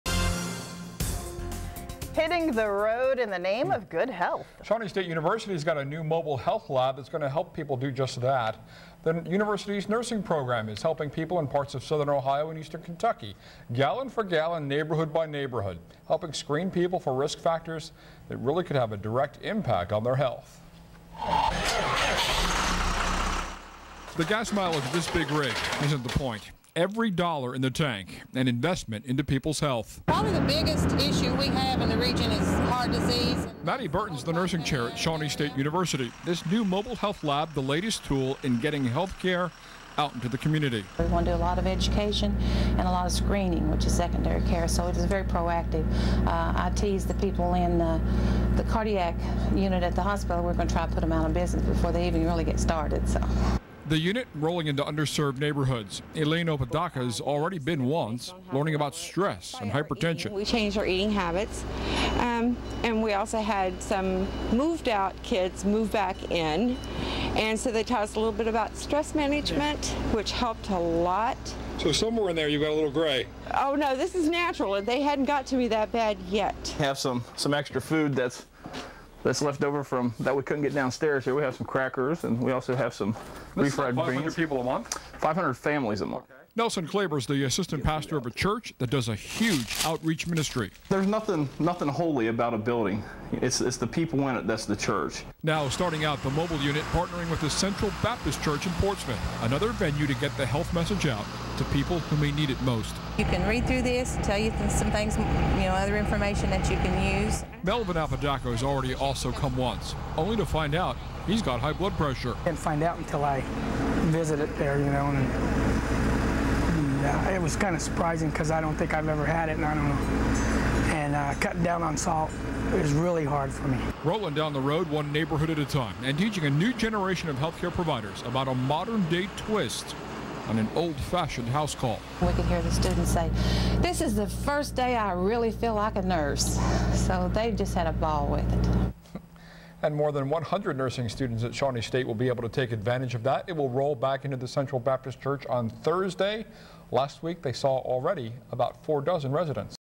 Shawnee State University Mobile Nursing Lab
The Mobile Health Unit is used to provide health screenings and health education in the community